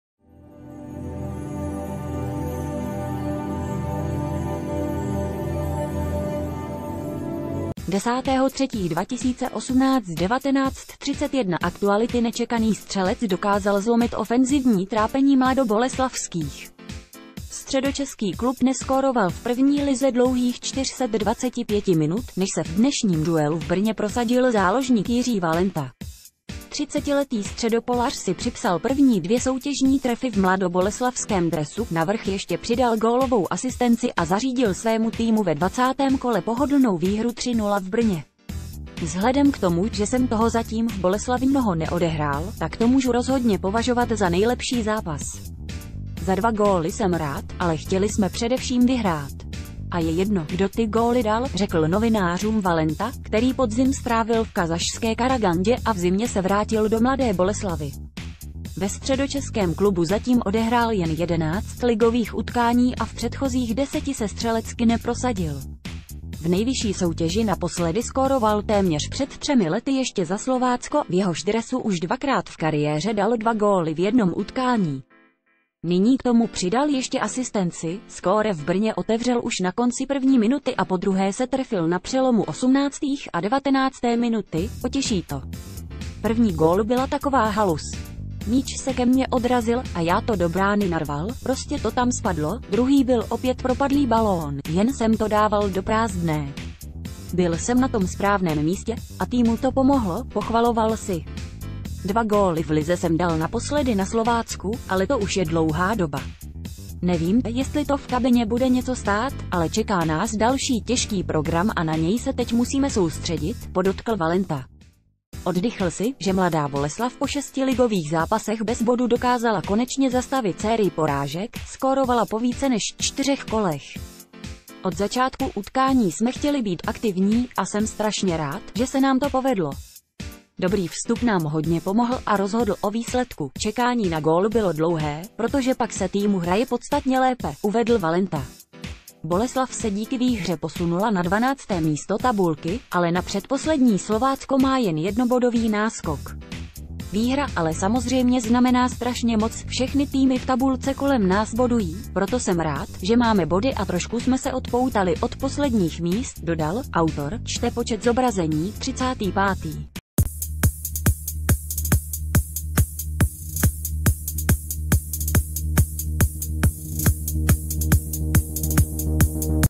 Rozbíjející se zprávy | Valenta v lize poprvé skóroval za Boleslav a ukončil její tápání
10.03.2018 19:31 - Aktuality Nečekaný střelec dokázal zlomit ofenzivní trápení Mladoboleslavských. Středočeský klub neskóroval v první lize dlouhých 425 minut, než se v dnešním duelu v Brně prosadil záložník Jiří Valenta. Třicetiletý středopolař si připsal první dvě soutěžní trefy v mladoboleslavském dresu, navrch ještě přidal gólovou asistenci a zařídil svému týmu ve 20. kole pohodlnou výhru 3:0 v Brně. "Vzhledem k tomu, že jsem toho zatím v Boleslavi mnoho neodehrál, tak to můžu rozhodně považ...